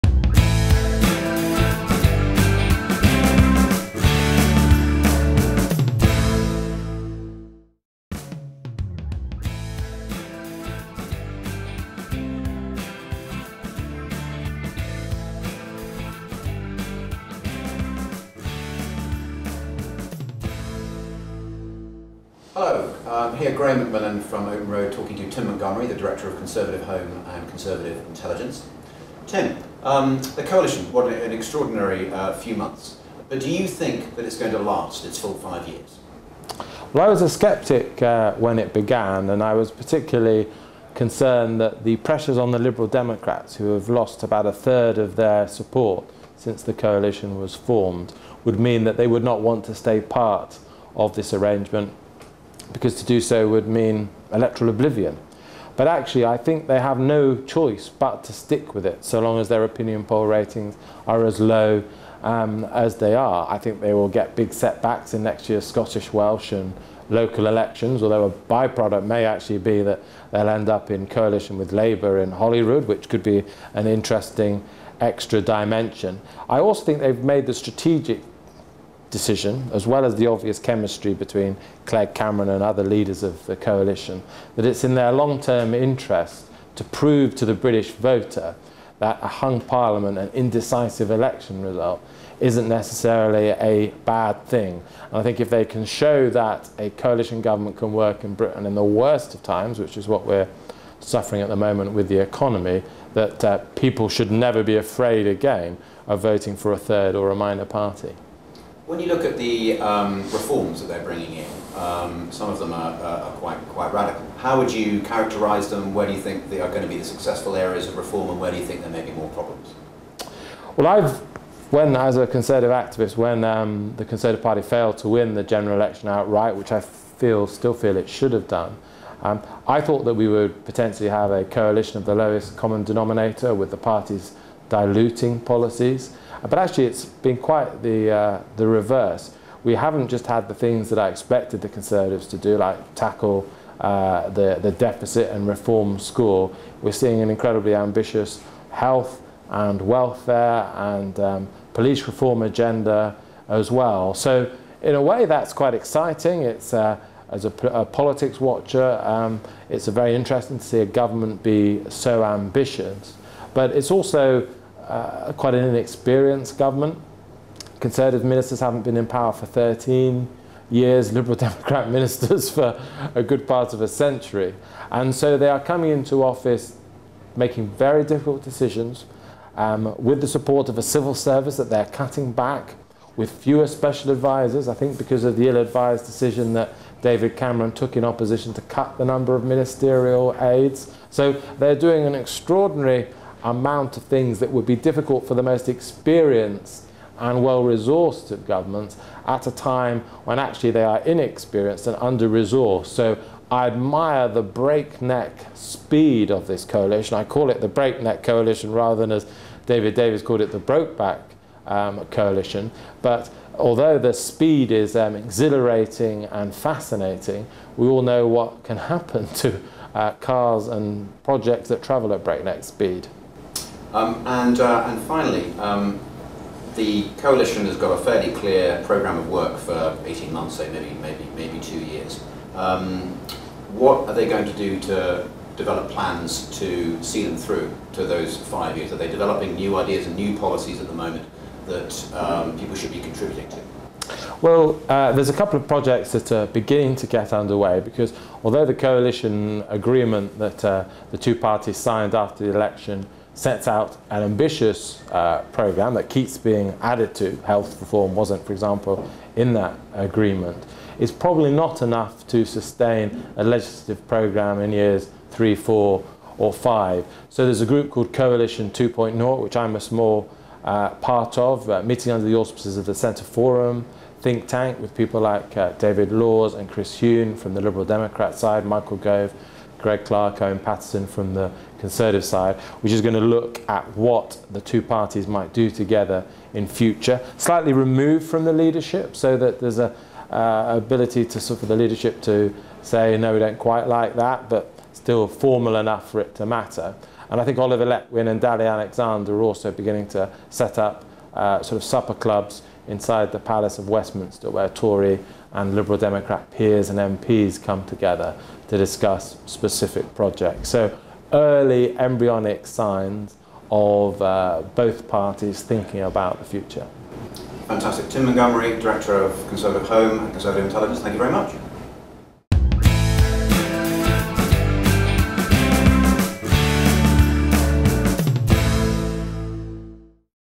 Open Road speak to Tim Montgomerie Founder & Editor of Conservative Home
Open Road speak to Tim Montgomerie Founder & Editor of Conservative Home at their Lunch Event on the on the 18th Novemeber 2010.